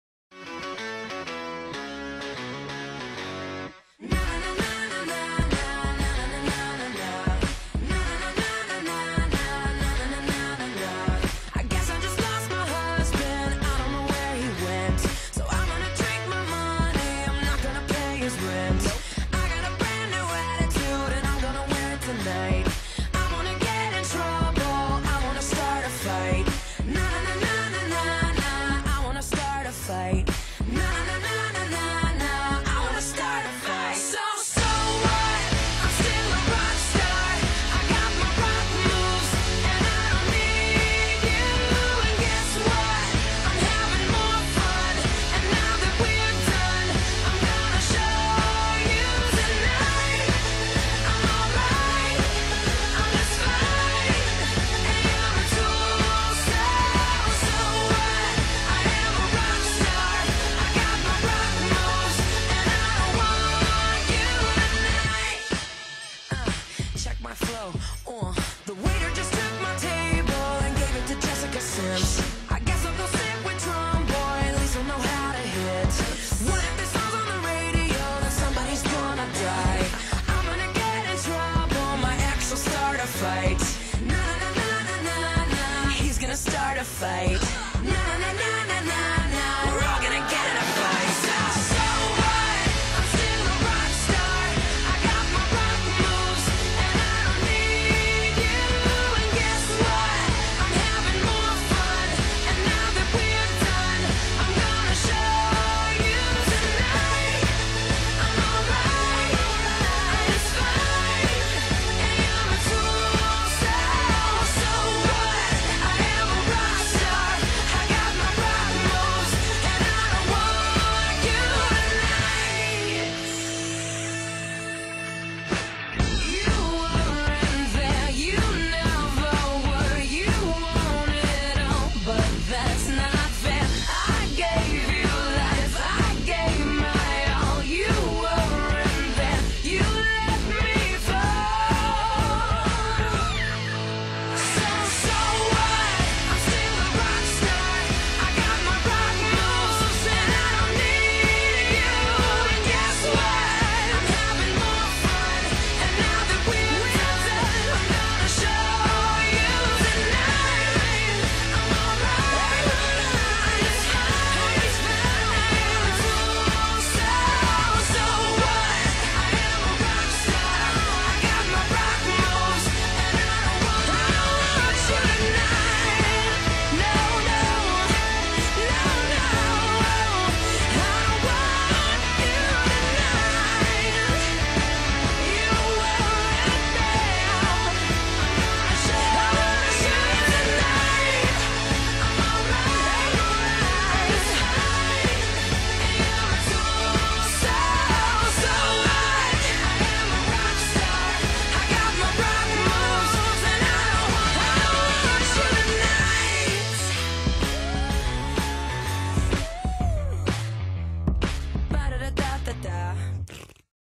A So What Transformation
I've seen others do similar videos, and I needed something to focus on to get my mind off my emotional darkness -- what better way than with makeup?? :)